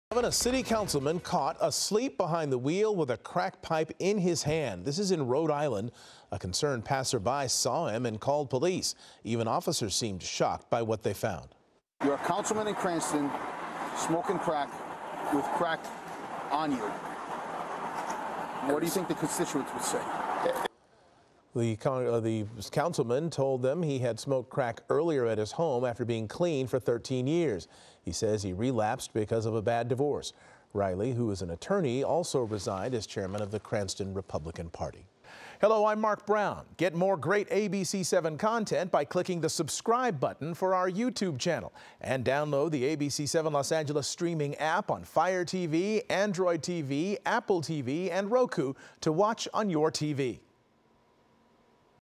Councilman arrested after getting caught asleep behind the wheel with crack pipe in hand
A Rhode Island city councilman seen on police body camera video getting arrested for drug possession has stepped down from his seat after he was caught asleep behind the wheel with a crack pipe in his hand. MORE